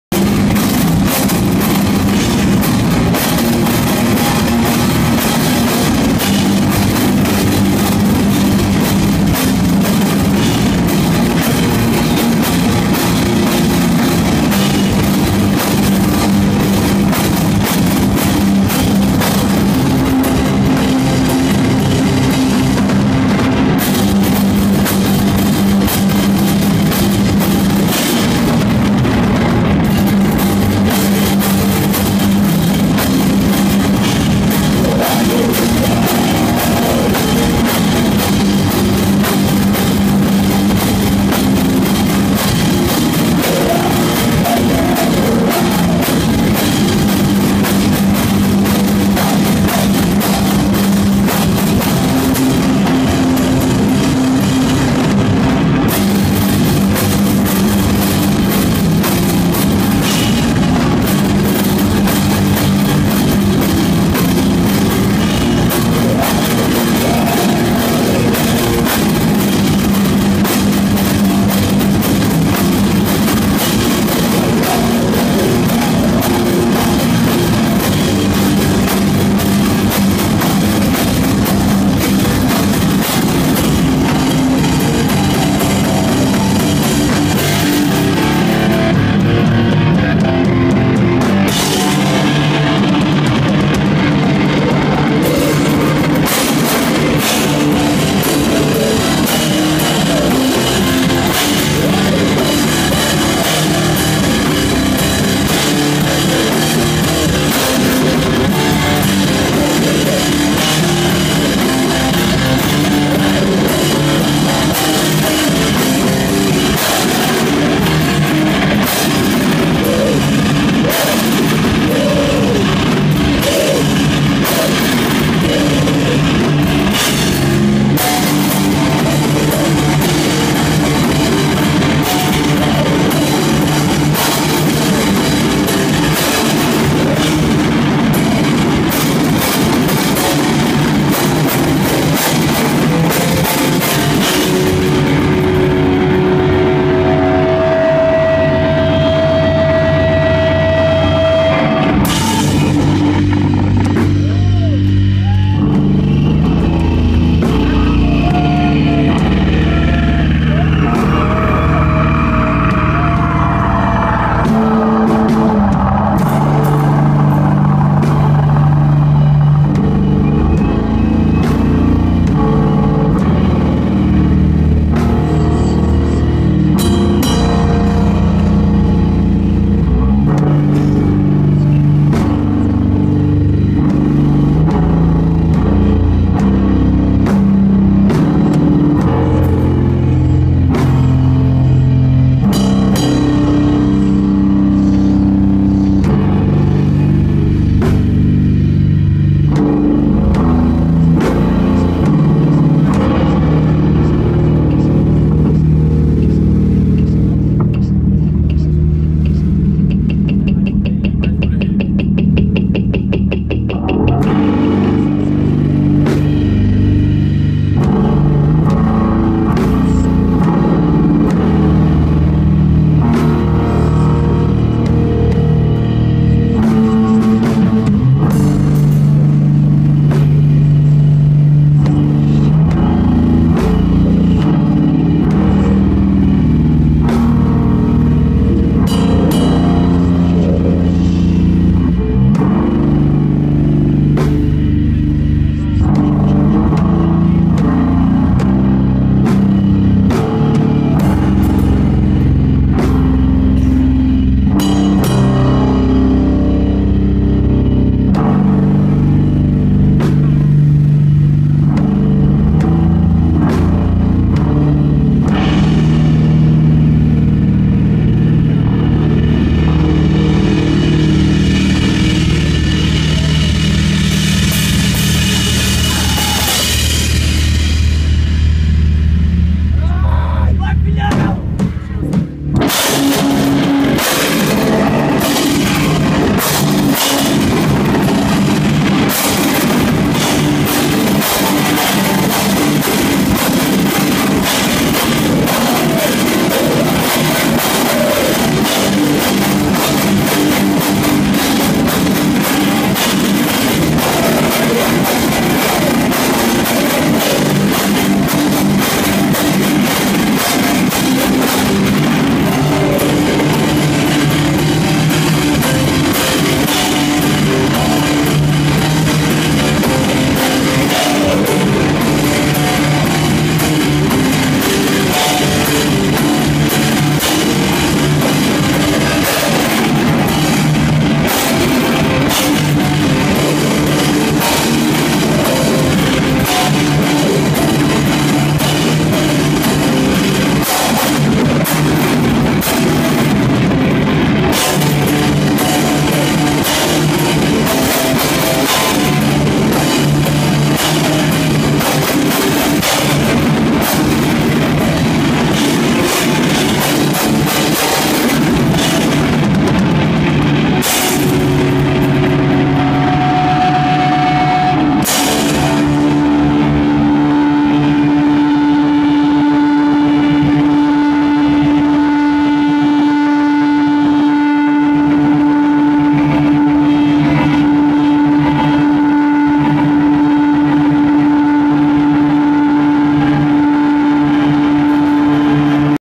La Casa Fantom @ Disgraça (2)
La Casa Fantom playing live @ Disgraça in Lisbon, Portugal (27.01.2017)